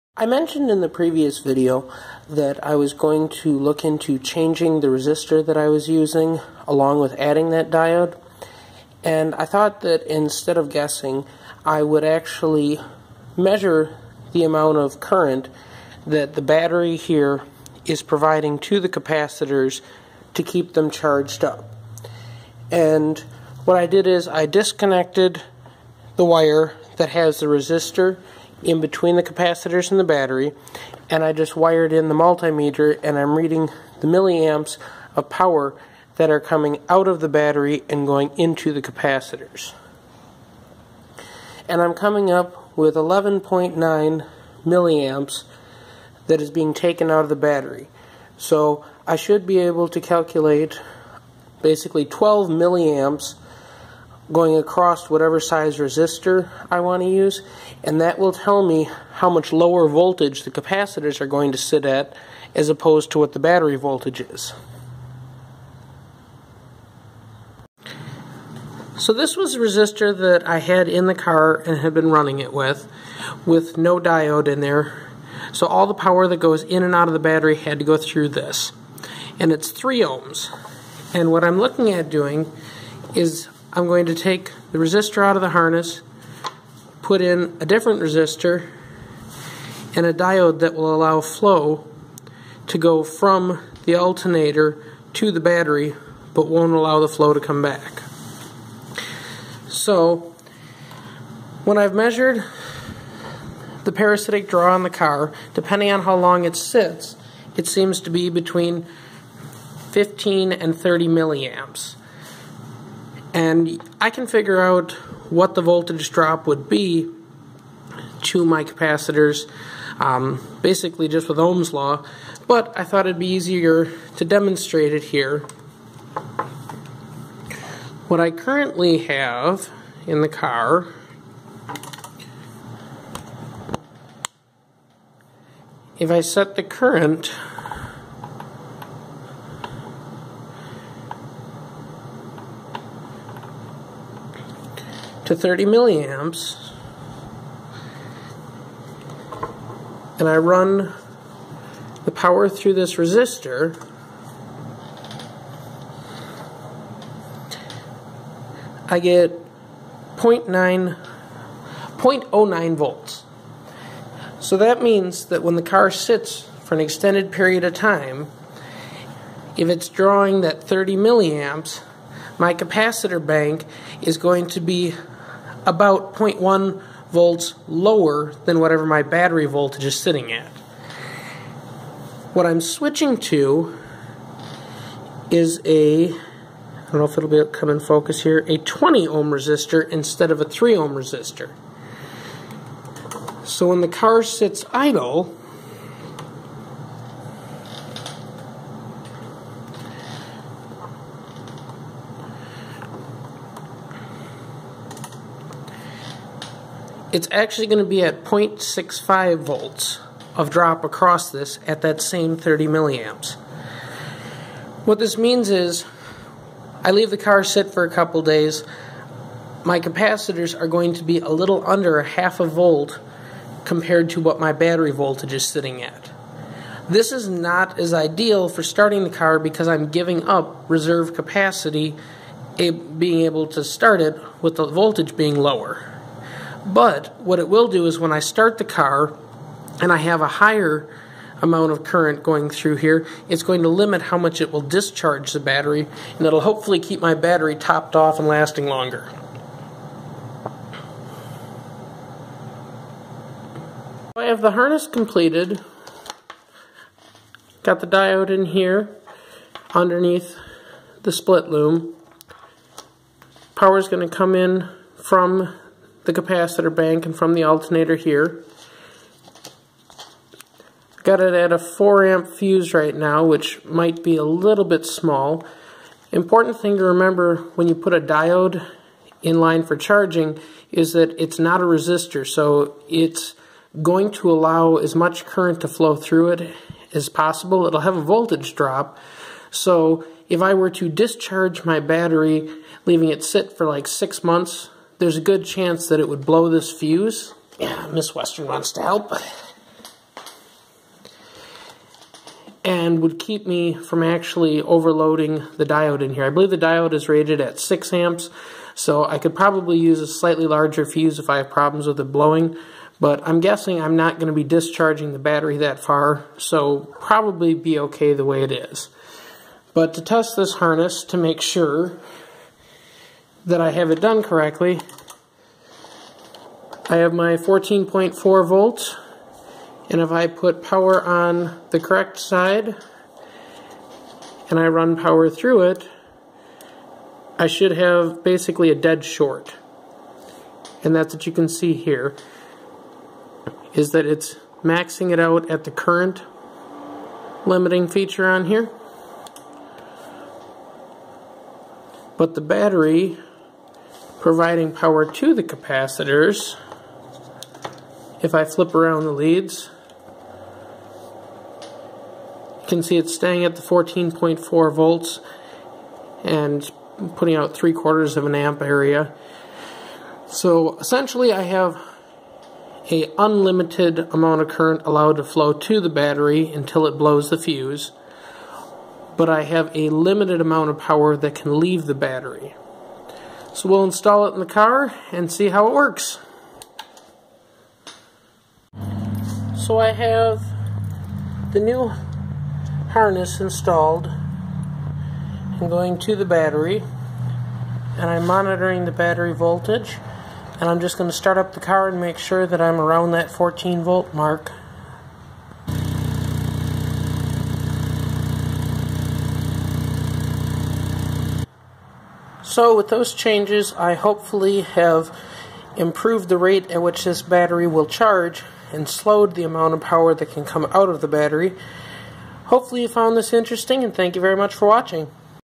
Using a super capacitor as a battery (part 12) Adding a diode
After using the super capacitors for a month I found that my small battery was getting down to 12.3 volts after a week of use. In this video I go over how I will be changing the system to fix this problem.